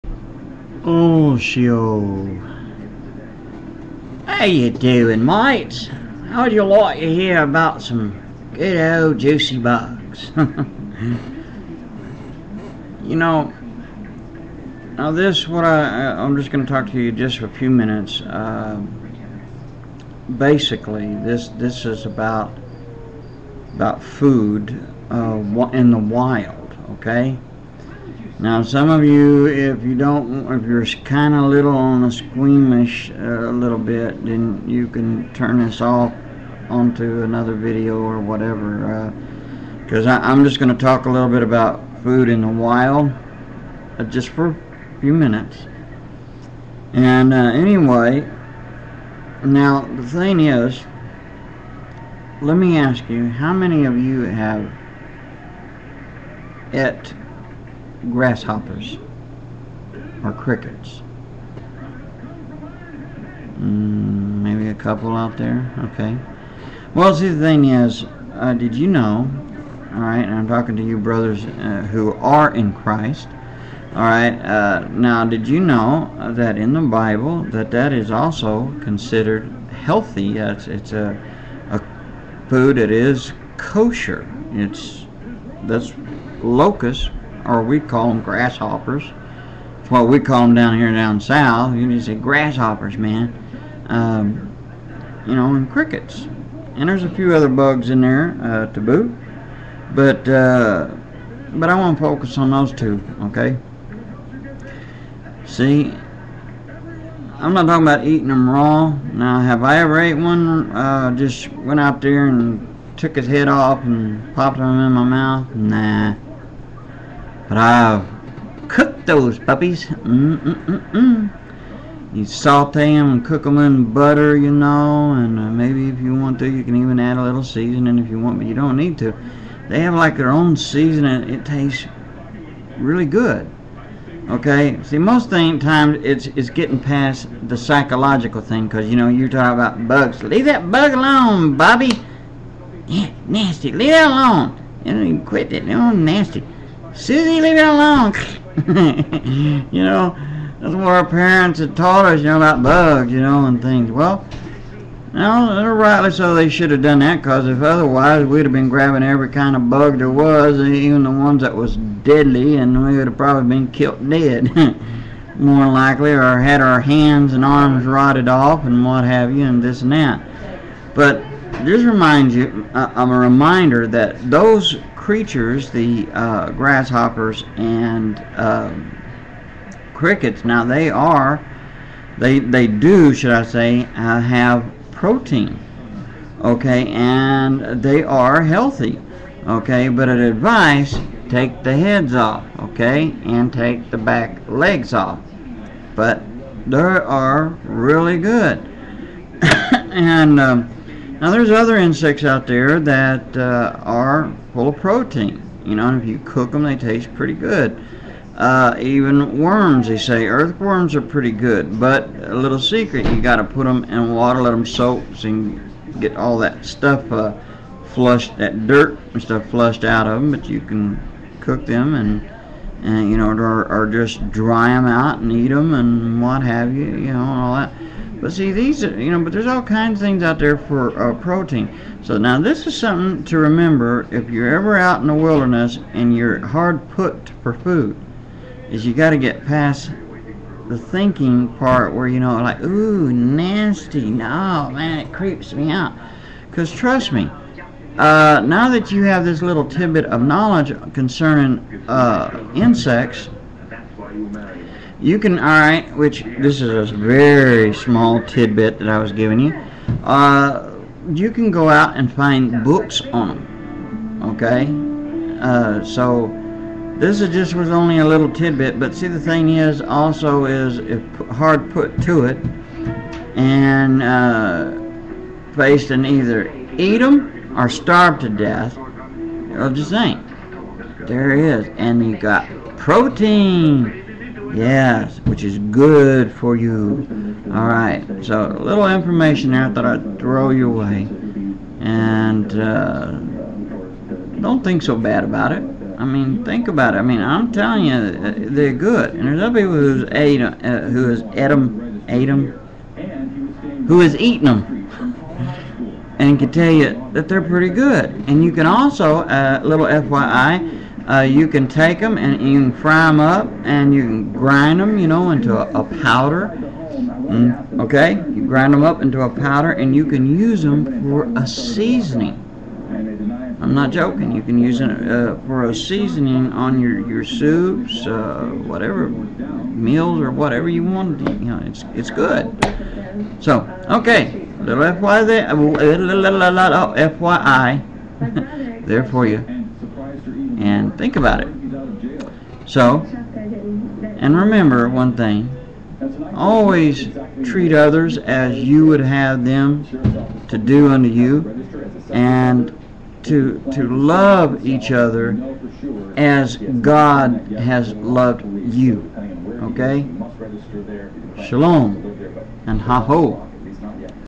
Wild Food in the Wilderness - December 30, 2009
This is a little tidbit about insects that are good for you as a food source if you are out in the wilds. If you want good info, on survival, check out wilderness outfitters.